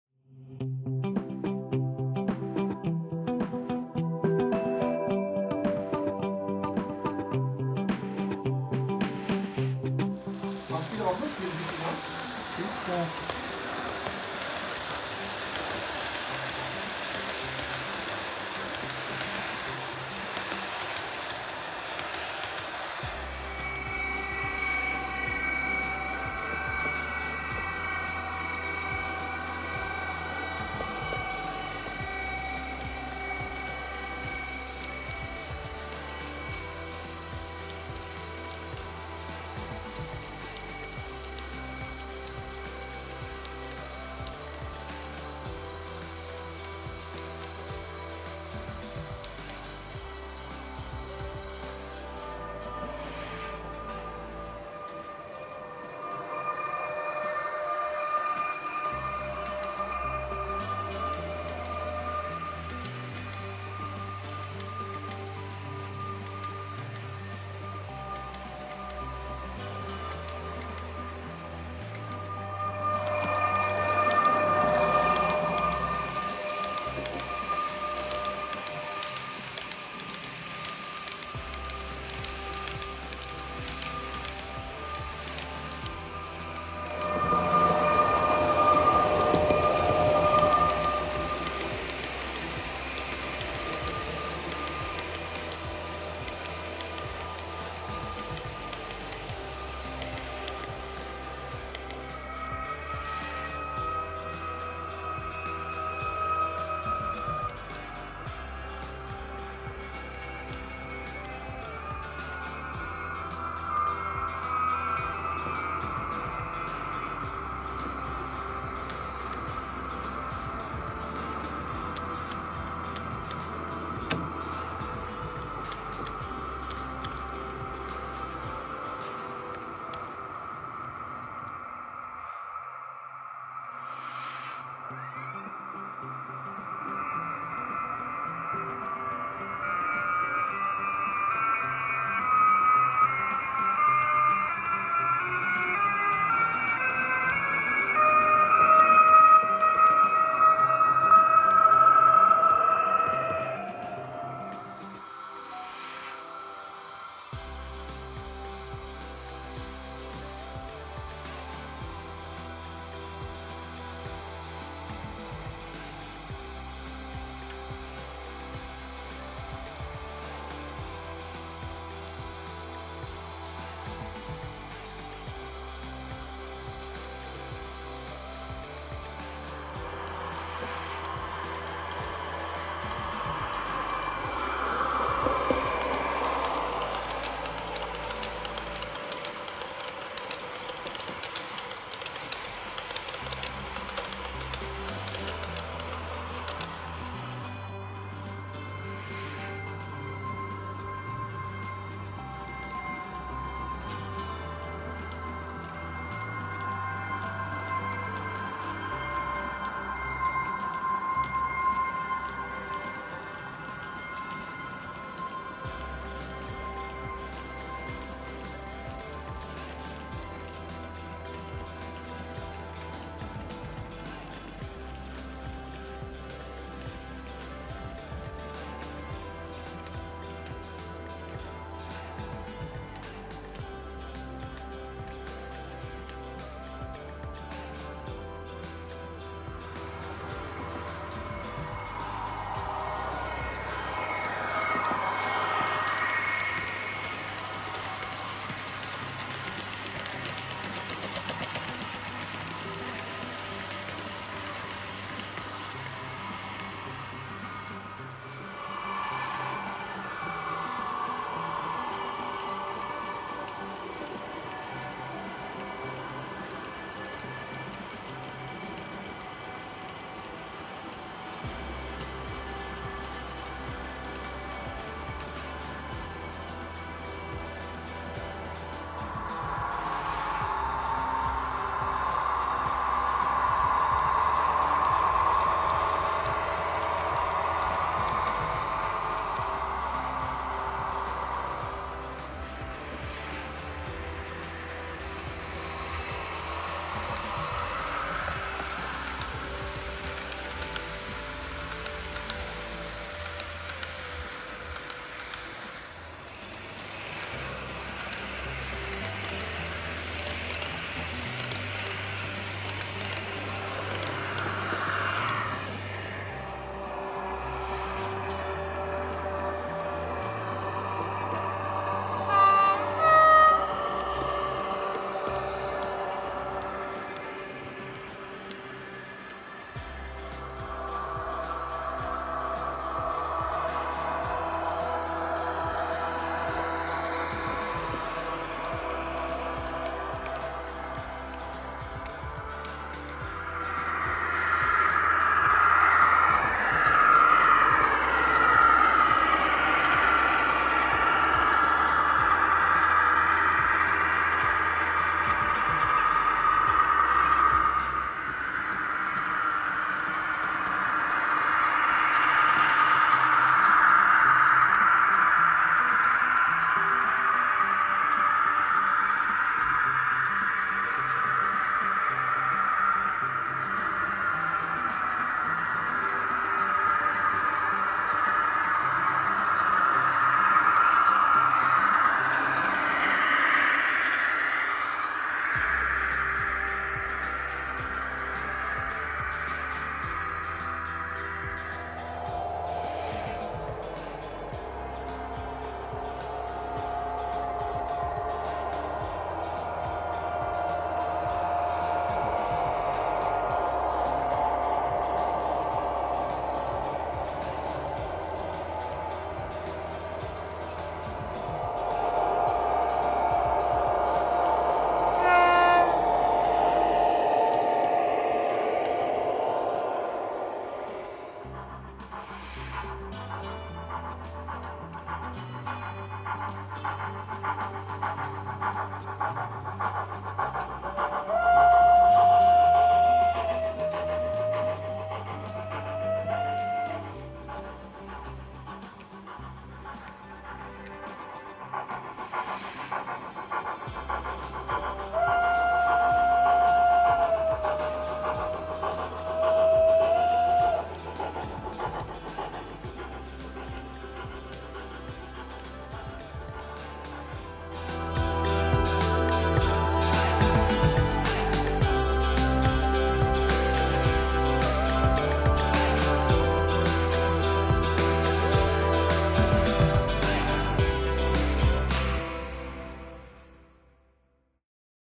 Modelleisenbahn H0 - Gastzüge auf Börni´s Modellbahn!!
Hallo,                                                                                                                                                                                                                                        Da ja momentan auch etwas an meiner Anlage gebastelt wird zeige ich Euch einstweilen ein paar Gastzüge die bei mir zu Gast waren!!                                                                                                                 Viel Spaß beim zuschauen und vielen Dank an die vielen Besucher die bei mir schon zu Gast waren und Ihre Züge auf die Schienen stellten!                                                                                                                          LG Börni